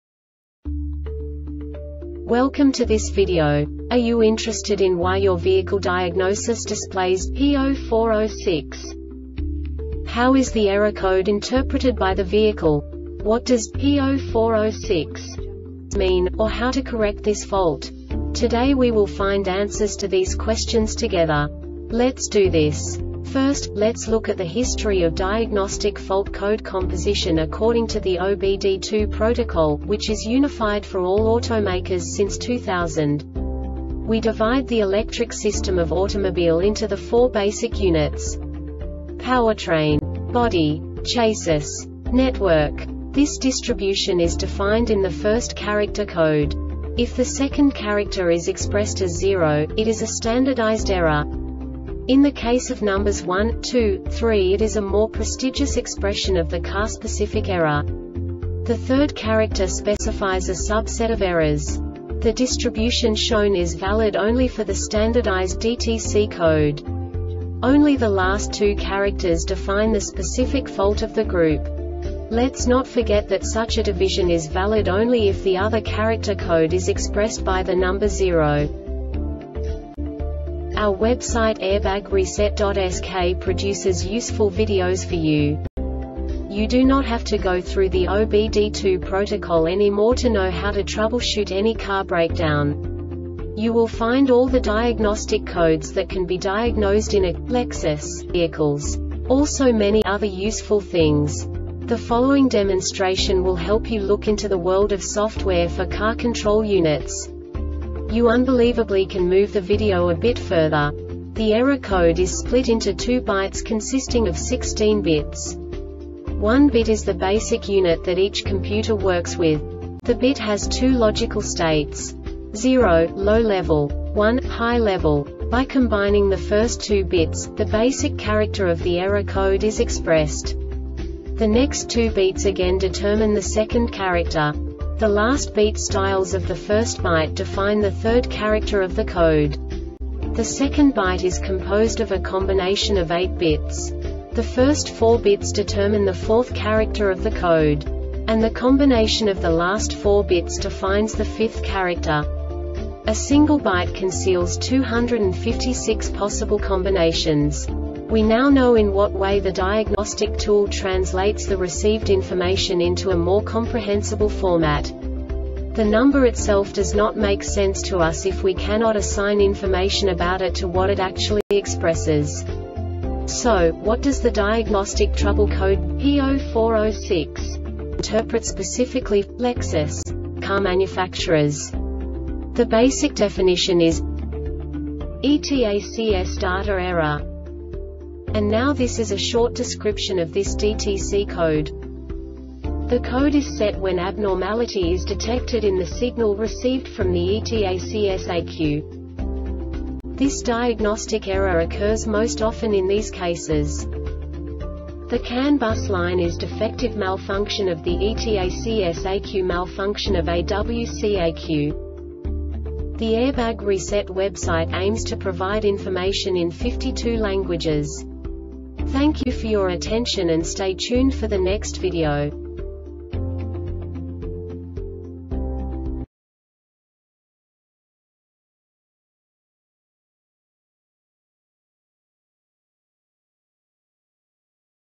DTC Lexus P0406 Short Explanation
Contents:
0:21 Basic DTC analysis according to OBD2 protocol standard.
1:48 Insight into programming
2:58 A diagnostically understandable description of the Diagnostic Trouble Code
3:05 Basic error definition

Make:
Lexus
Code:
P0406
Definition:
ETACS Data Error
Description:
The code is set when abnormality is detected in the signal received from the ETACS-ECU.
Cause:
 The CAN bus line is defective. Malfunction of the ETACS-ECU Malfunction of AWC-ECU